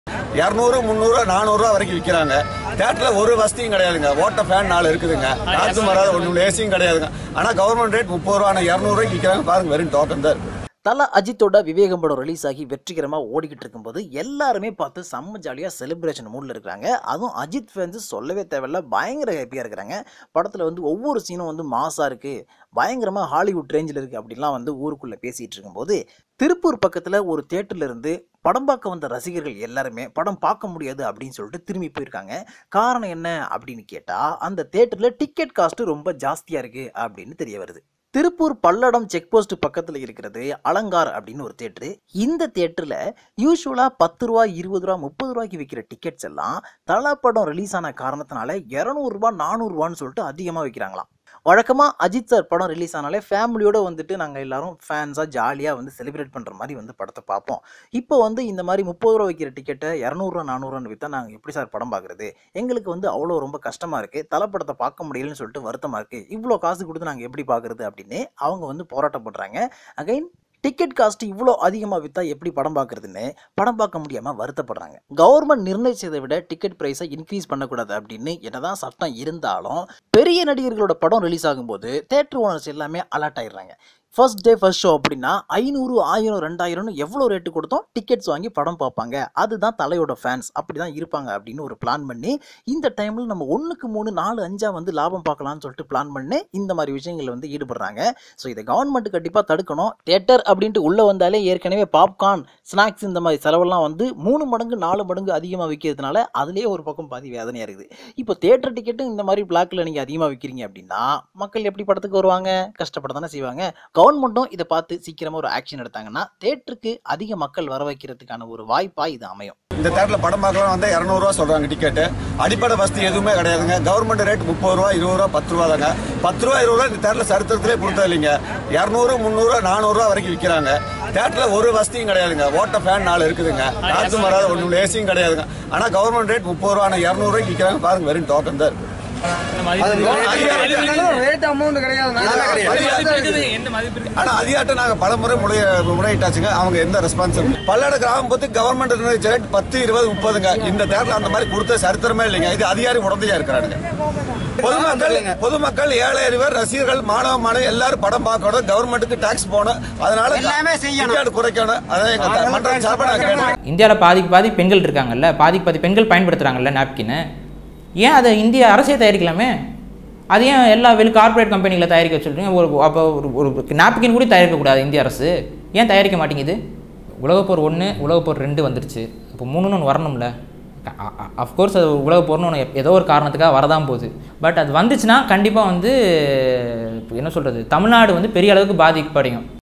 விவேகம் டிக்கெட் விலையால் மக்கள் அதிர்ச்சி | Shocking Ajith Vivegam Ticket Cost | Latest News
Ajith Vivegam movie which was released recently is a super hit, but recently in Tirupur people have protested that will not watch the movie due to the exorbitant ticket cost.